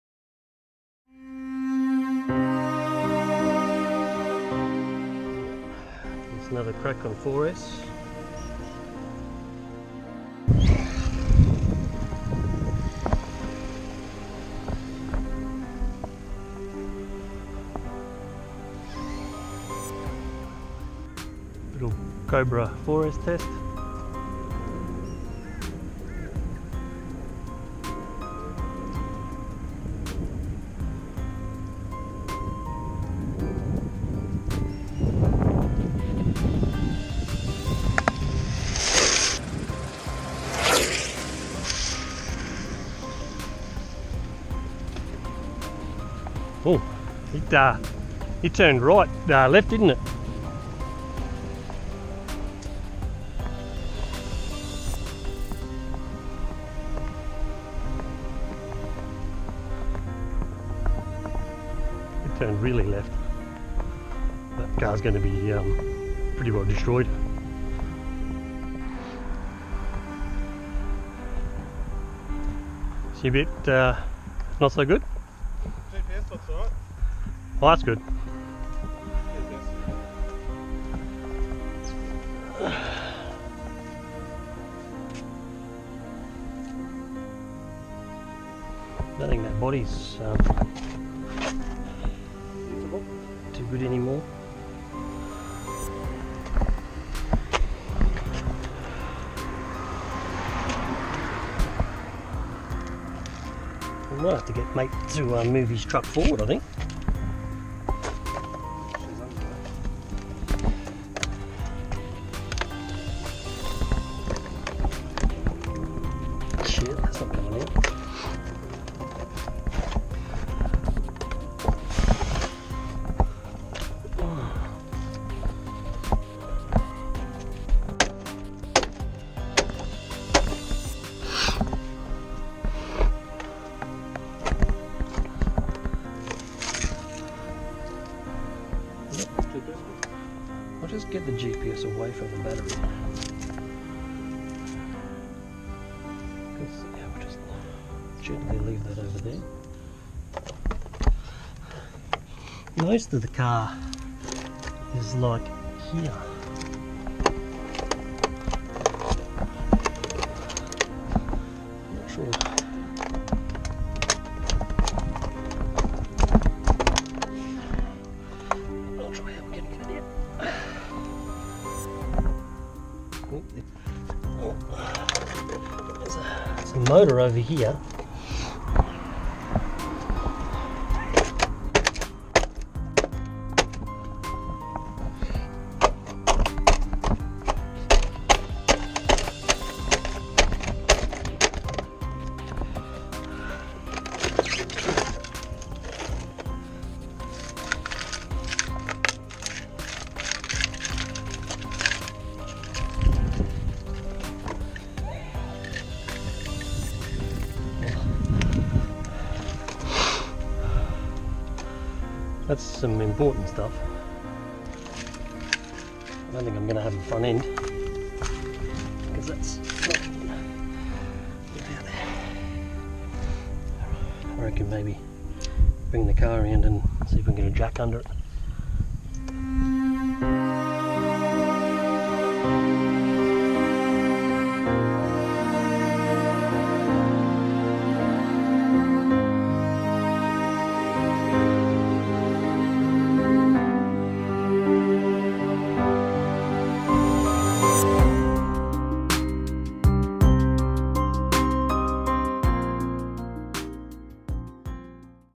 Worst crash ever!!!
This will have to be the worst crash I have had in during my time with RC's.

It came down to a foam tire failure. I used some old foams I had lying around thinking I was only going to be testing 2s.

I should have got new tires.